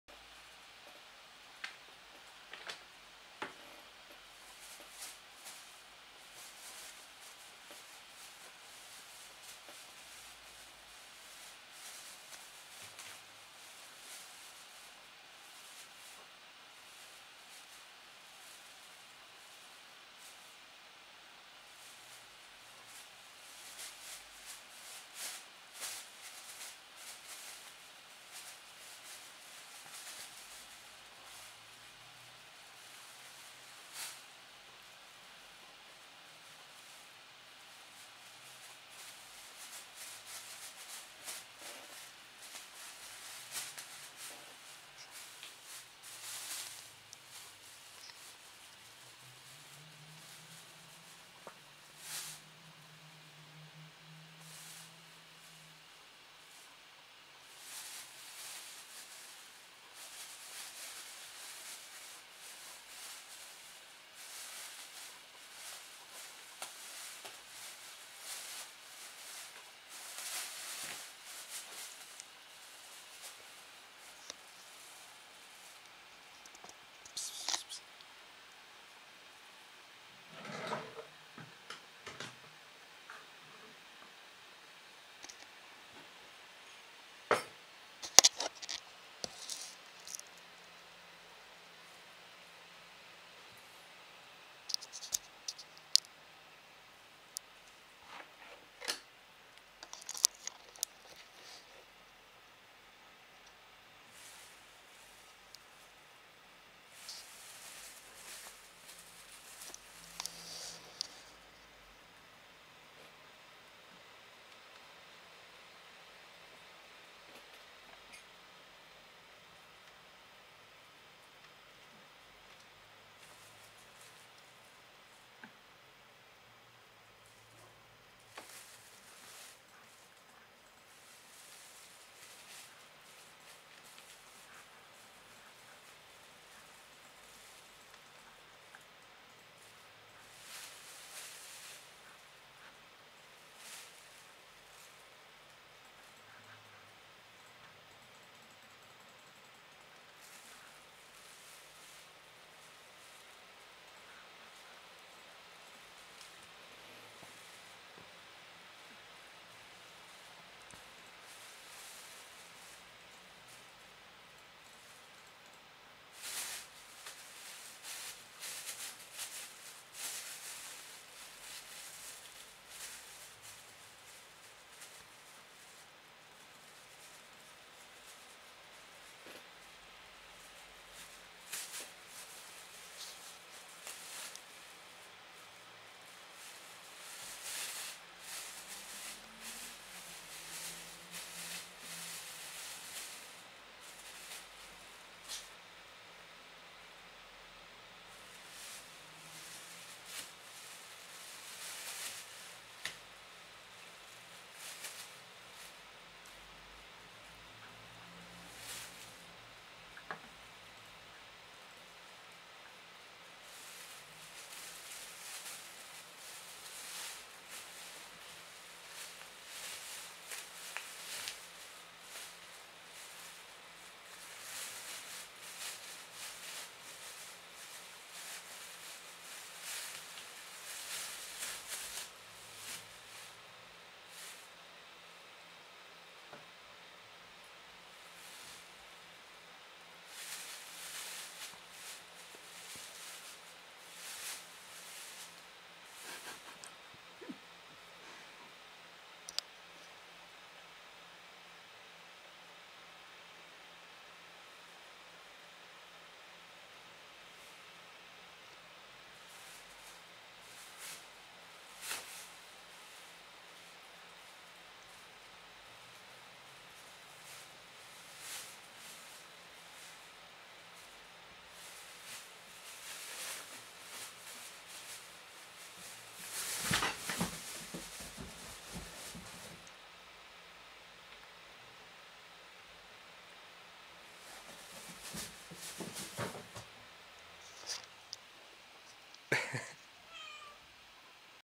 Kitten VS a plastic bag
Kitten VS a plastic bag. My kitten has grown up! I picked her up from the street and I even have a video of bringing her home the first time. This time, she was exploring a plastic bag. To not get an injury, I was monitoring her throughout filming.